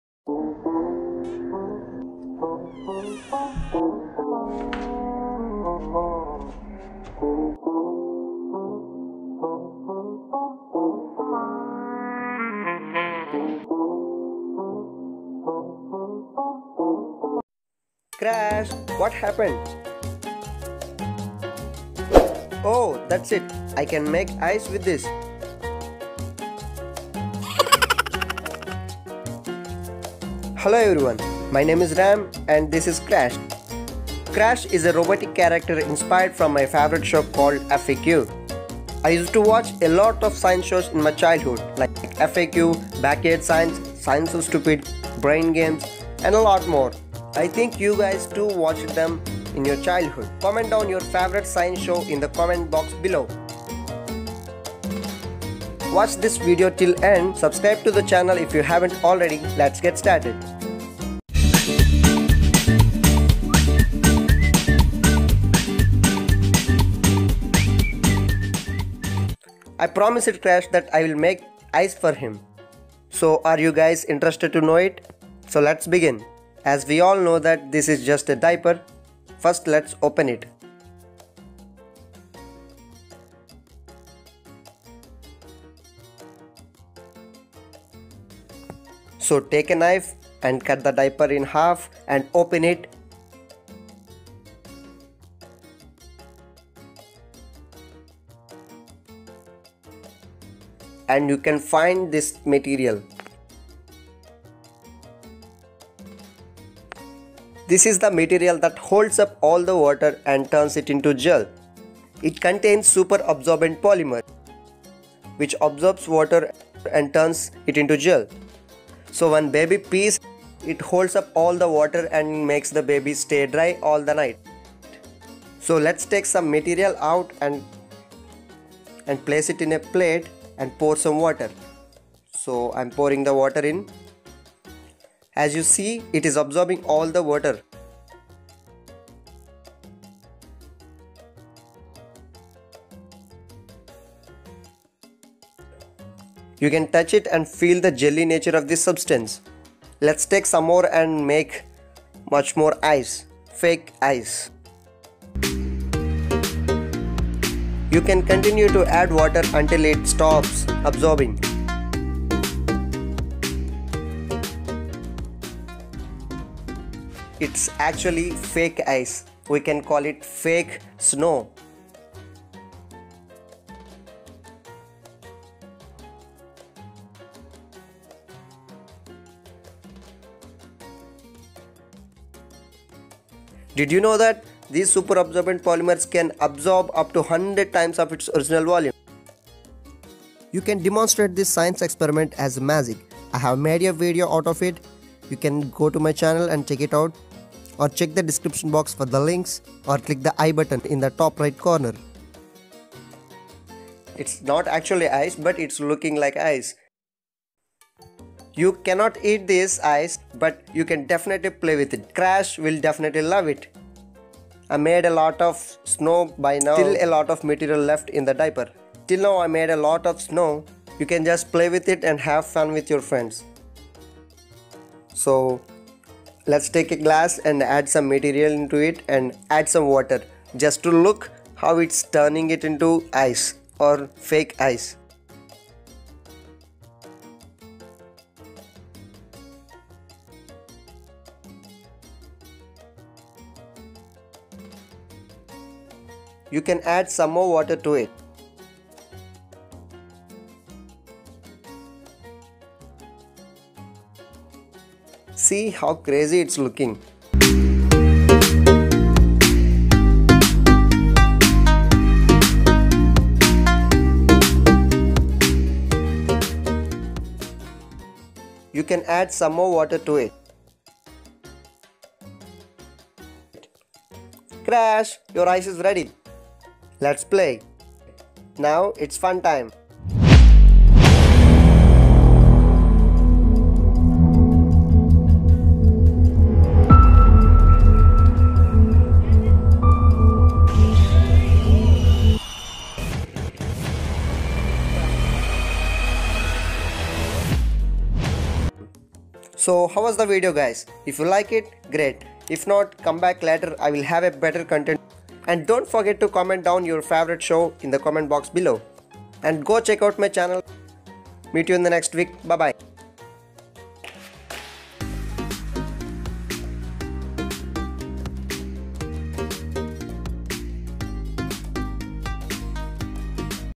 How to make fake snow at home || how to make snow balls || fake ice
In this video i will tell you how you can make fake snow balls or fake ice at home very easily with science.
watch the video to know how to make fake ice that you can play with.
Making snow balls at home DIY magic of science

✅freeze water into ice in seconds

LinkedIn – Ramakrishna gudivada 🔸🔸𝗚𝗲𝗮𝗿 𝗜 𝘂𝘀𝗲
🔸Mobile (Main camera)
🔸Keyboard & mouse